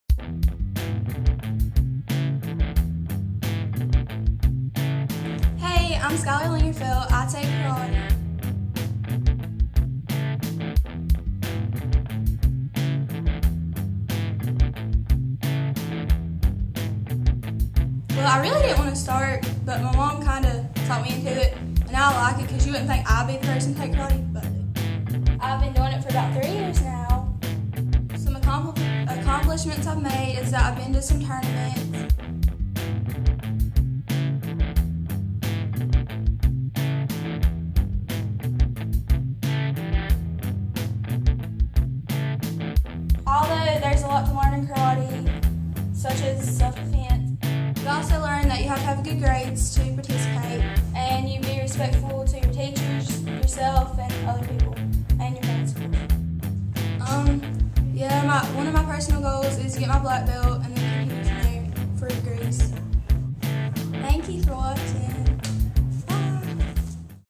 CECHS Student Spotlight Skylar
As part of our 9th Grade Student Spotlight series at CECHS, this video spotlights Skylar Lingerfelt and her accomplishments in karate. Skylar has been taking karate for 3 years and has competed in tournaments.  It is her goal to become a 3rd degree blackbelt.  We are proud of our students and all of their successes, and this is another way we celebrate them. Enjoy.